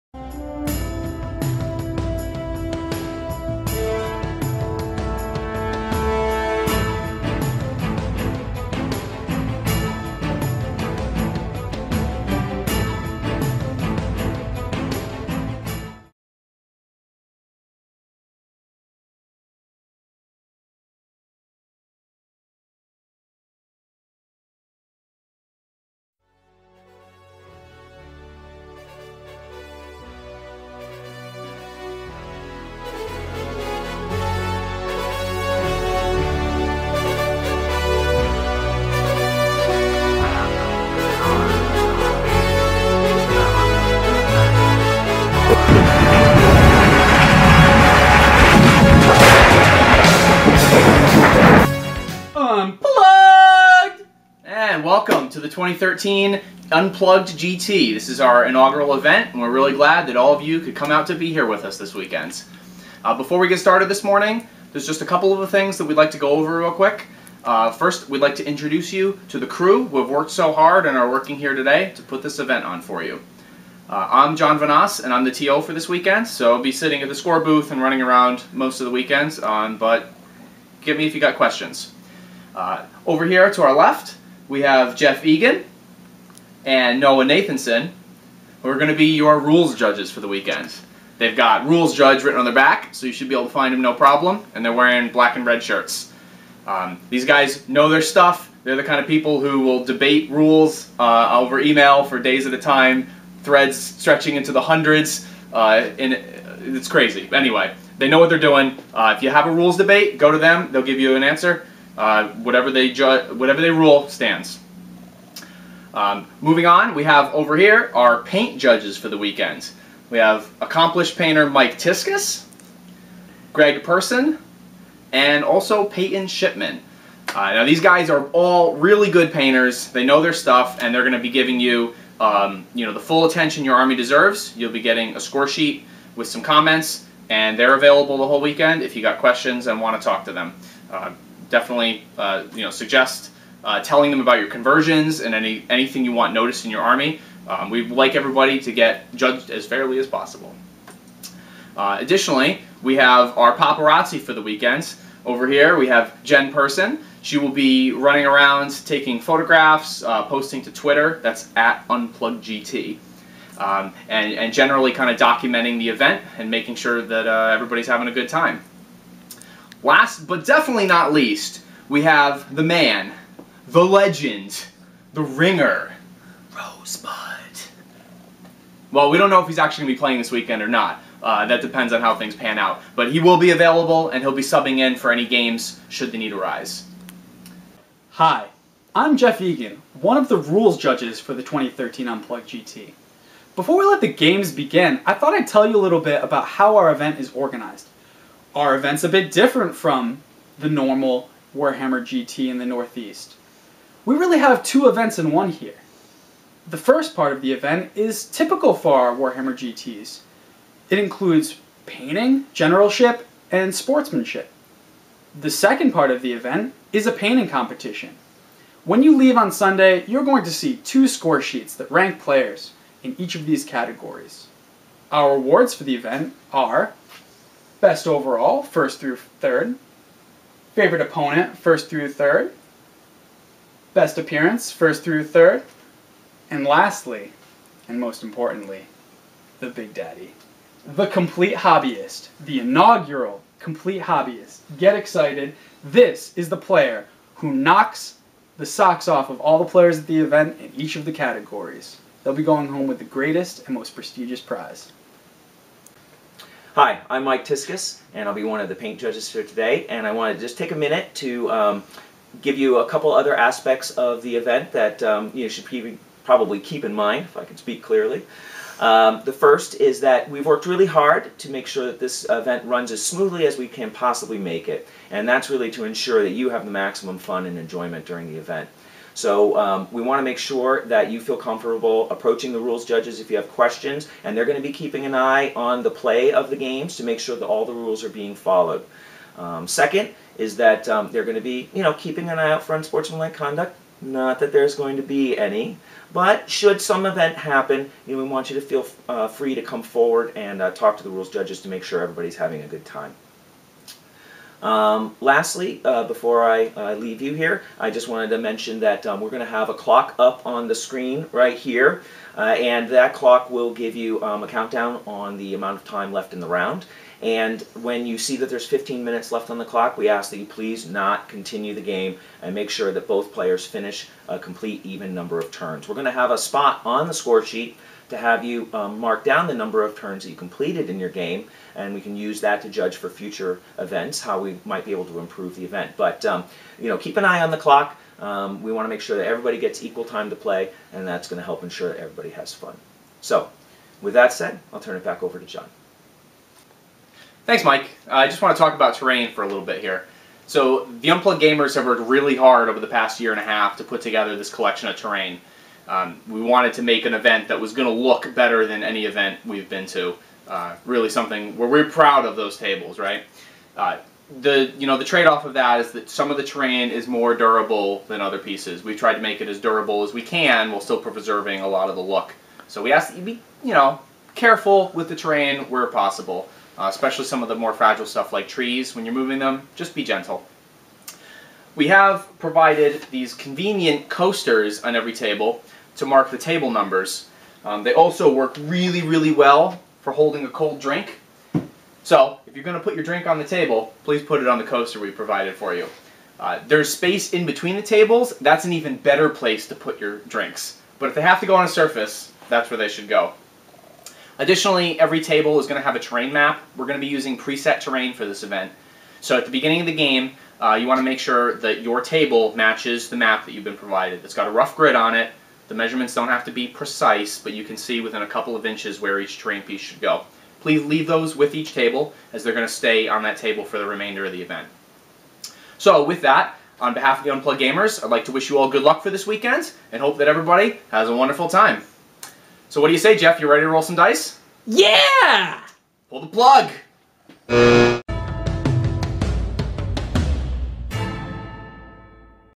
TerranScapes - Unplugged GT Intro Video
This was the introduction video we produced for the Unplugged Gt.  It was presented via LCD projector on a screen.  Mostly, this is my way of showing off how much we tried to present a professional image for the event.  :)